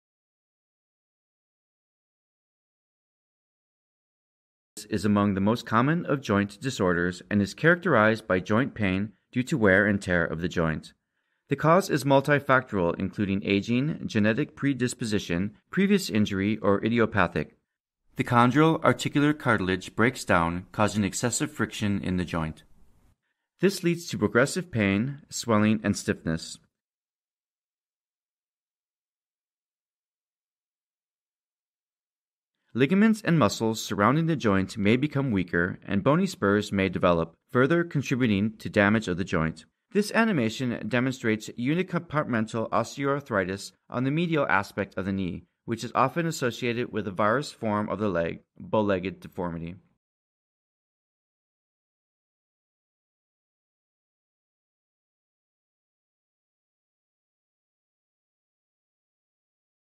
Medial Compartmental Osteoarthritis
Osteoarthritis (OA)
Knee OA  causes cartilage to be lost (medial loss occurs in 70% of cases). The ligaments become lax and the joint becomes less stable, with local pain arising from the ligaments and tendons.

The image at the end of video is an x-ray. Shows that the tibiofemoral joint space in the medial compartment of both knees is narrow, particularly on the left.